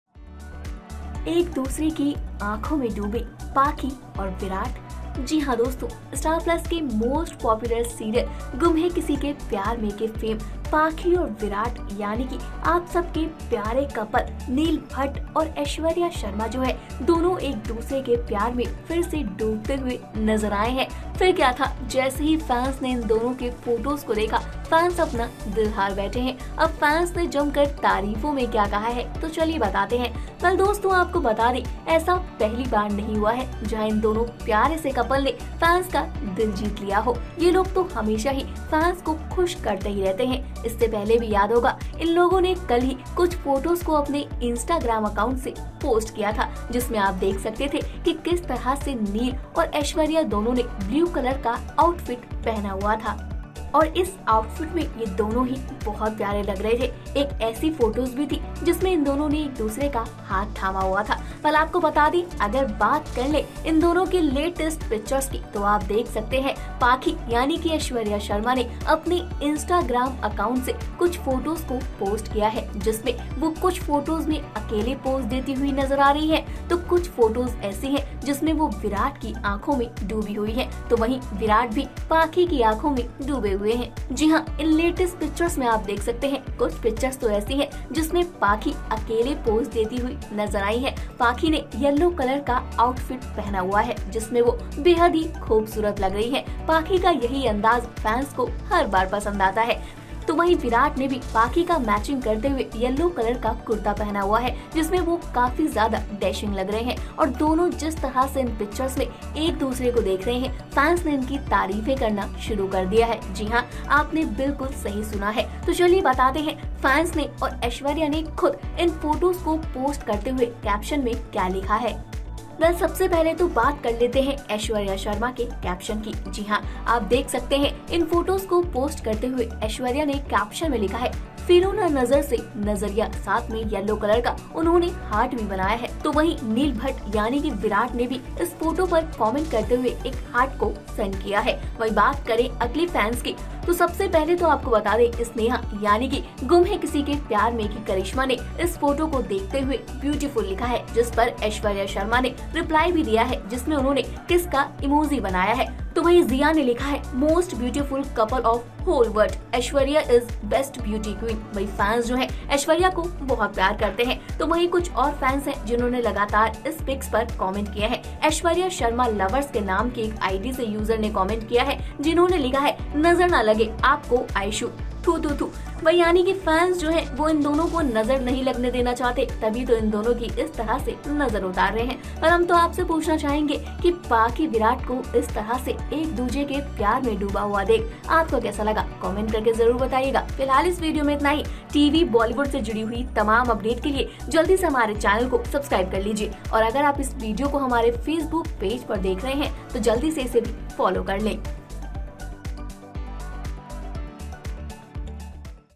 GHKKPM Fame Aishwarya Sharma और Neil Bhatt का प्यार देख Fans हुए खुश, Photos Viral
GHKKPM fame Aishwarya Sharma and Neil Bhatt shared pictures,Fans gets happy, Photos Viral. Aishwarya Sharma and Neil Bhatt make for one of the most handsome pairs in the TV world. They are seen in Gum Hai Kisi ke Pyar Mein as Pakhi and Virat. Neil Bhatt and Aishwarya Sharma doing fun in new fun videos.